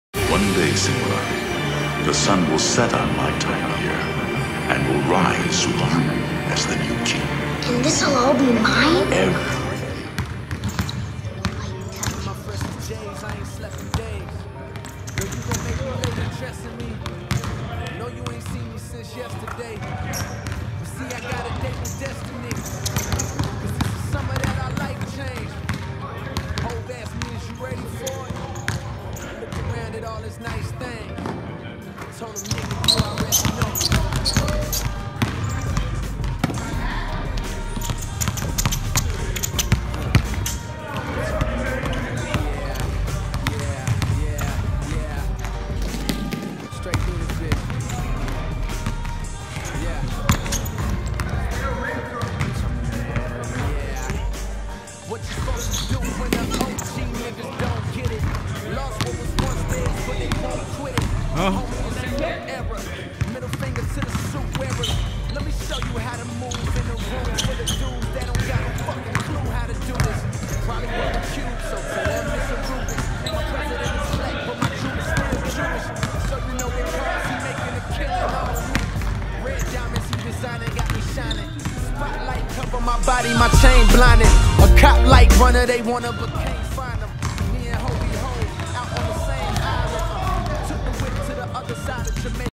Jermaine Taylor Workout With Skills Trainer Brian Macon
Watch Jermaine Taylor's Preseason workout as he prepares for the upcoming season